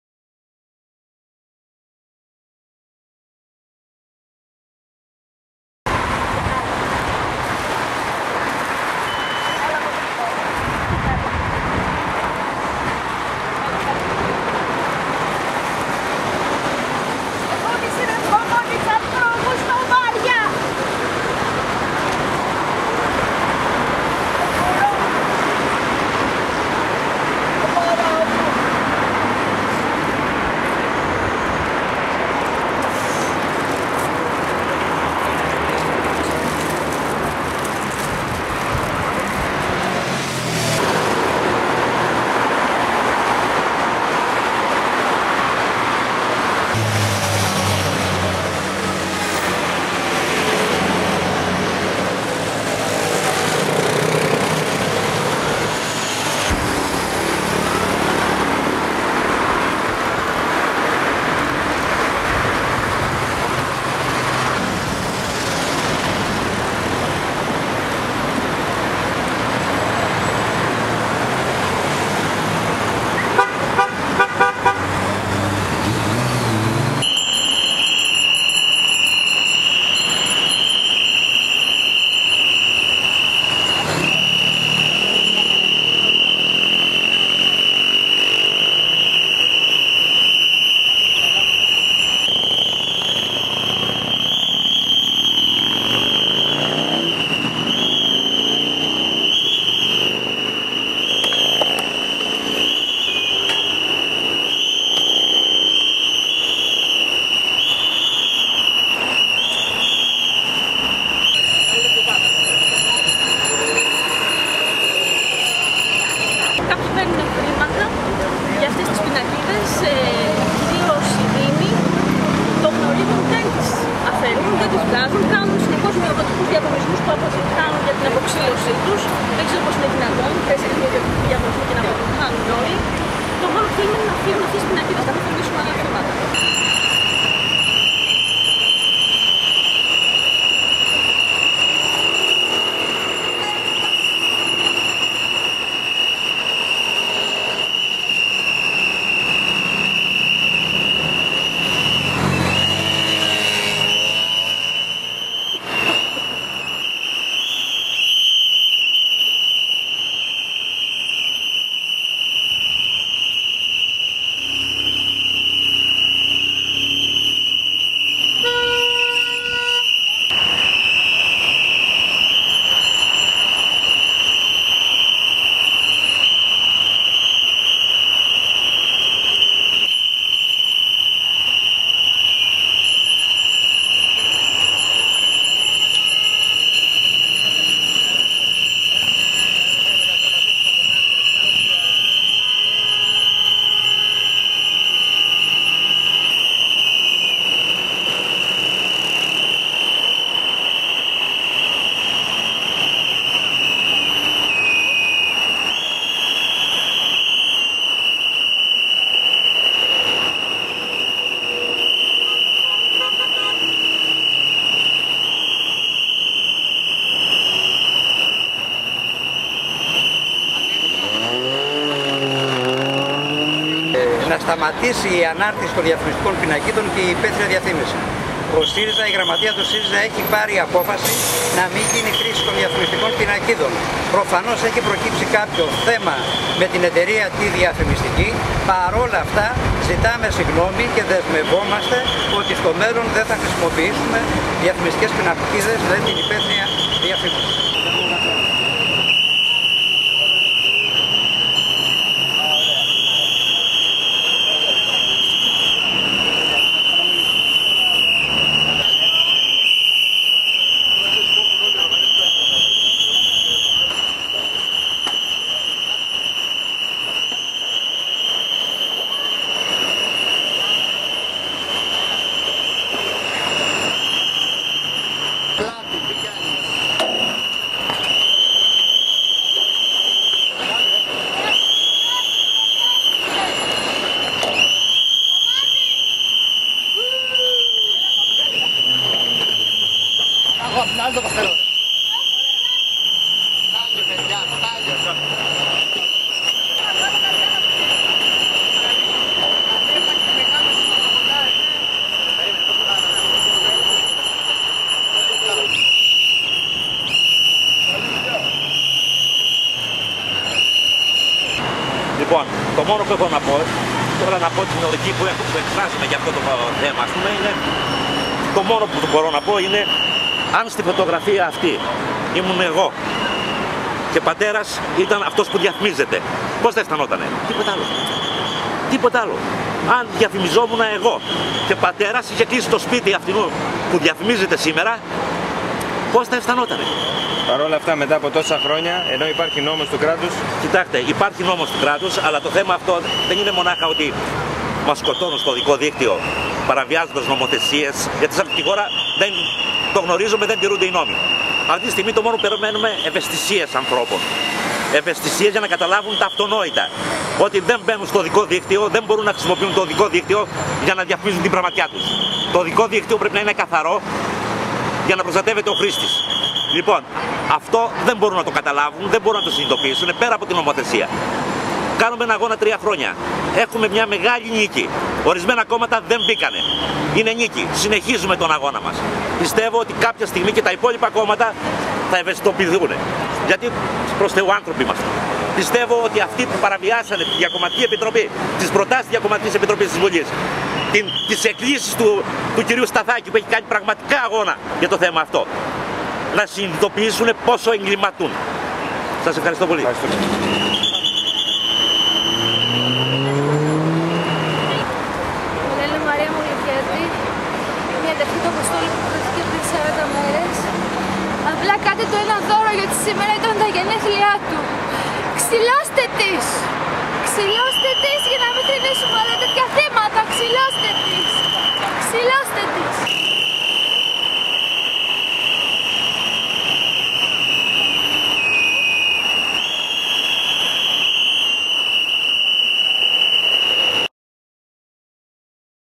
"Ξηλώστε τις!"
Με αφορμή το θάνατο του 21χρονου Αποστόλη Μολυβιάτη επάνω σε παράνομη διαφημιστική πινακίδα (24.08.09), συγγενείς, φίλοι, γονείς θυμάτων και ενεργοί πολίτες διαδήλωσαν την Παρασκευή 2 Οκτωβρίου, 2 ημέρες πριν τις εθνικές εκλογές, στη Λεωφόρο Κηφισίας, ακριβώς στο σημείο που έχασε τη ζωή του ο νεαρός. Μιλούν οι αδελφές του Αποστόλη Μολυβιάτη, Νόρα και Μαρία, ο Μανώλης Σταυρουλάκης, πατέρας του Γιάννη Σταυρουλάκη, που έχασε τη ζωή του με τον ίδιο τρόπο σε παράνομη διαφημιστική πινακίδα και ο κ. Κώστας Φωτεινάκης, υποψήφιος βουλευτής του ΣΥ.ΡΙΖ.Α στη β΄Αθηνών.